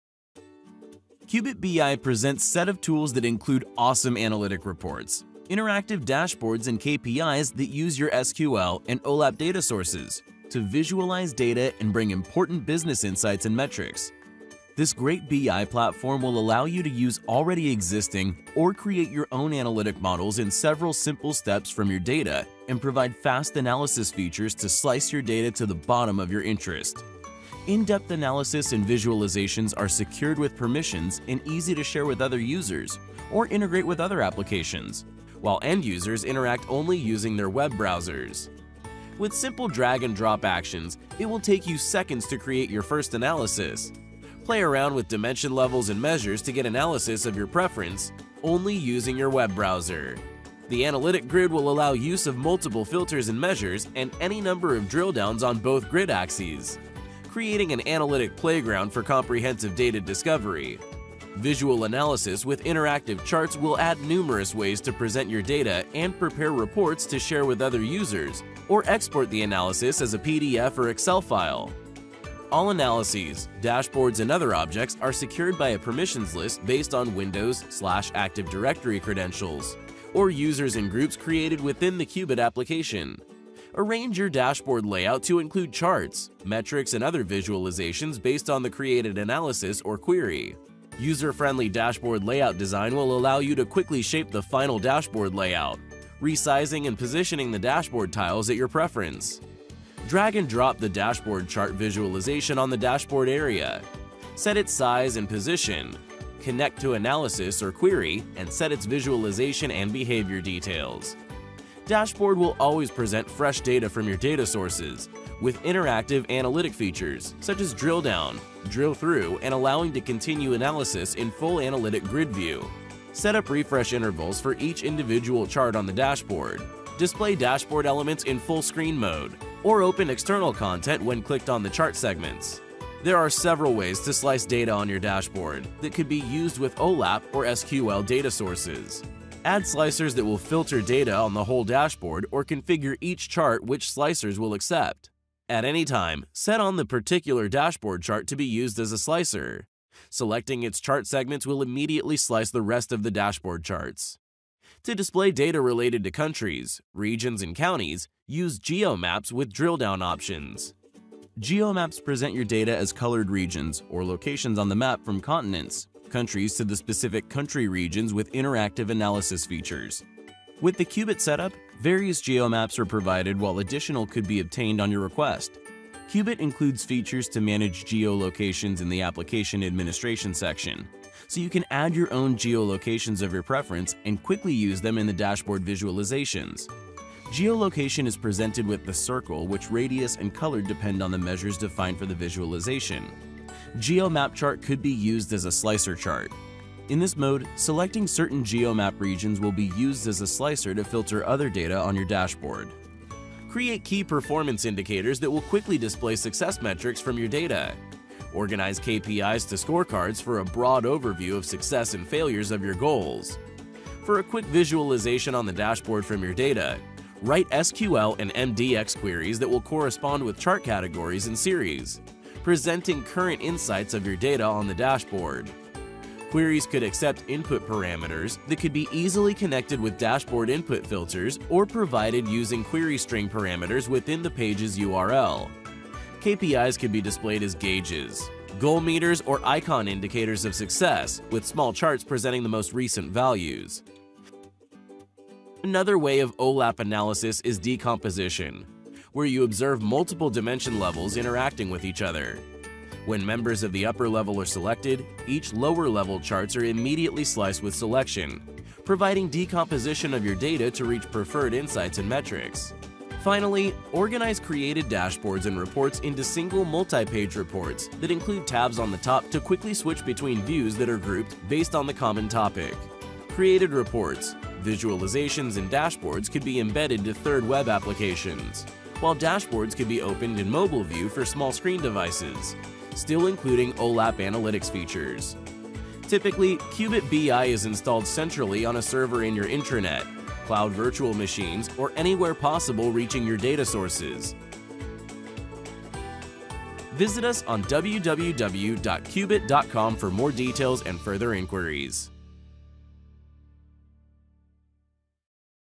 Easy BI tools with Kyubit
This easy-to-adopt BI platform will skyrocket your data analysis abilities.
Learn about Easy BI tools with Kyubit Business Intelligence. Overview of the most cost-effecitve Business Intelligence, Analytics and Dashboards features with Self-Service BI, OLAP, and SQL data visualizations and analysis. OLAP/SQL data analytic Dashboards and Reports created in minutes, secure collaboration, interactive analysis, scheduled subscriptions, embedded BI features, and multitenancy support! Kyubit BI platform offers a free personal license and inexpensive commercial license options.

00:00 Introduction
00:40 Analyze data and create reports
01:22 Create Dashboards
02:15 Dashboard Slicers
02:36 Geo maps
03:25 KPIs, Gauges and Scorecards
03:40 SQL and MDX query visualization
04:06 Decomposition Analysis
04:25 Multipage reports
04:35 BI environment

Collaborative web-based BI platform that is straightforward to implement and use. Kyubit BI uses your OLAP, SQL, and CSV data sources to deliver fast and beautiful Self-Service BI reports and dashboards that could be sliced and drilled-down to the details of your interest. Analytic features are available for your existing OLAP databases or Analytic Models created in Kyubit from your SQL and CSV data. BI content could be secured and shared, embedded to other applications or used for scheduled subscriptions.